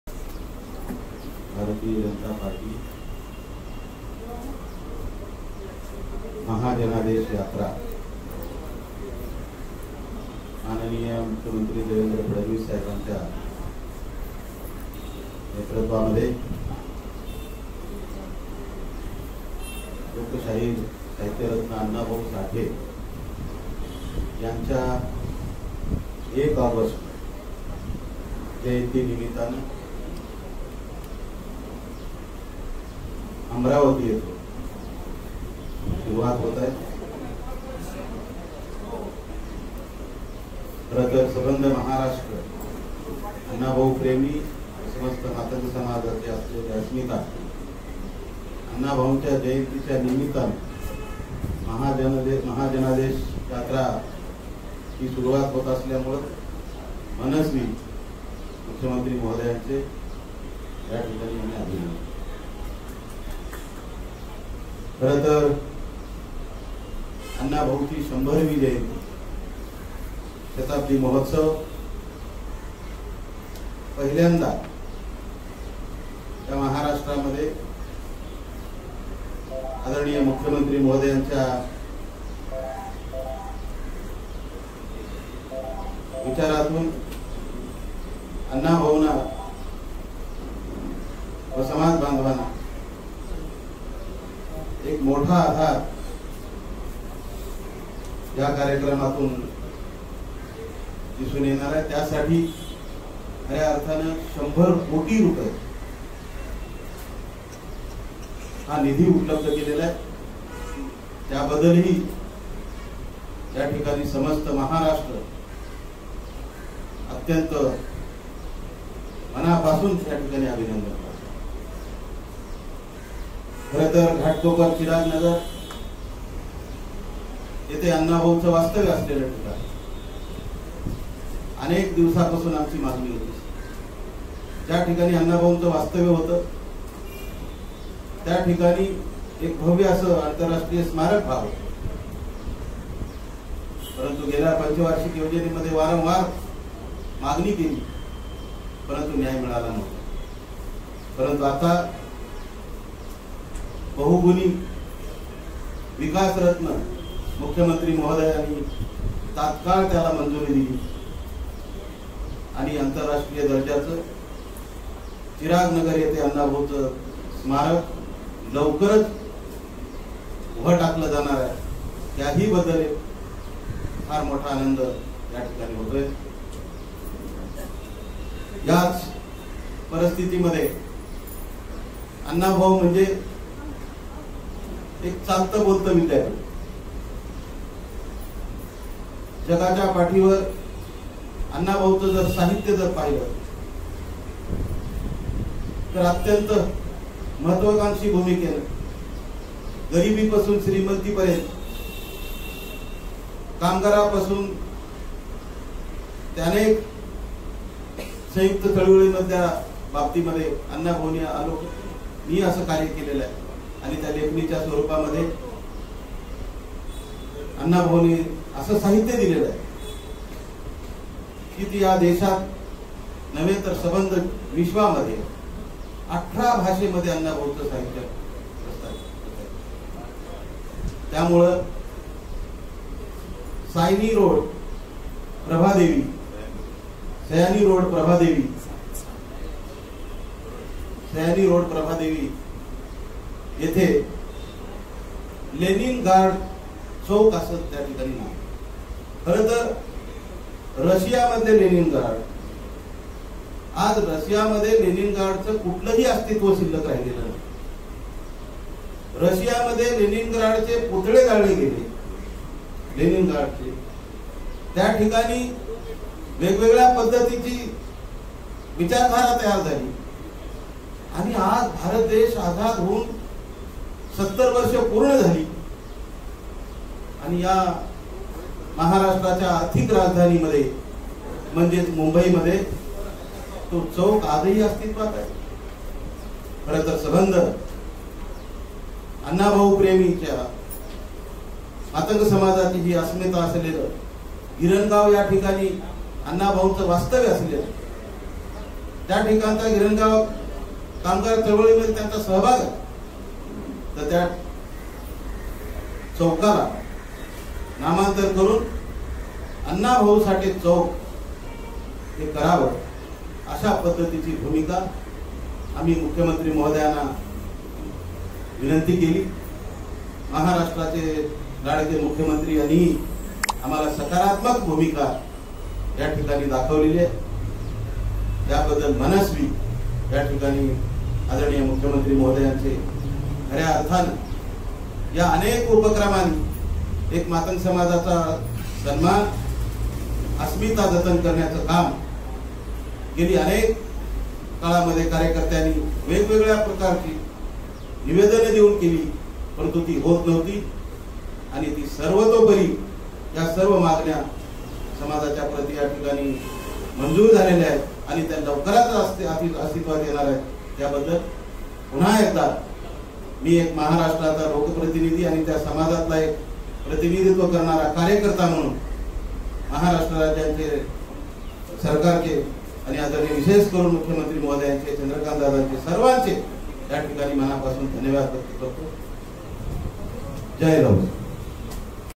लेनीनग्राड चौकाचे नाव बदलून लोकशाहीर अण्णाभाऊ साठे यांचे नाव द्या- आ. सुधाकर भालेराव
‌मुंबईतील प्रभादेवी येथील लेनीनग्राड चौकाचे नाव बदलून त्यास लोकशाहीर अण्णाभाऊ साठे यांचे नाव द्यावे अशी मागणी भारतीय जनता पार्टीचे आमदार सुधाकर भालेराव यांनी सोमवारी मुख्यमंत्री देवेंद्र फडणवीस यांच्याकडे केली. भाजपा प्रदेश कार्यालयात आयोजीत पत्रकार परिषदेत ते बोलत होते.
सुधाकर भालेराव म्हणाले की, “रशियामध्ये साम्यवादी विचार संपला असून लेनिनग्राडचे महत्त्वही संपलेले आहे. त्यामुळे लोकशाहीर अण्णाभाऊ साठे यांच्या जन्मशताब्दी वर्षाचे औचित्य साधून आपणही प्रभादेवी मधल्या सयानी रोड येथील लेनिनग्राड चौकाचे अण्णाभाऊ साठे चौक असे नामकरण करावे.”त्याचबरोबर, गिरणगावात अण्णाभाऊ साठे यांचे स्मारक विकसीत करावे अशी मागणी मुख्यमंत्री देवेंद्र फडणवीस यांच्याकडे करण्यात आली आहे.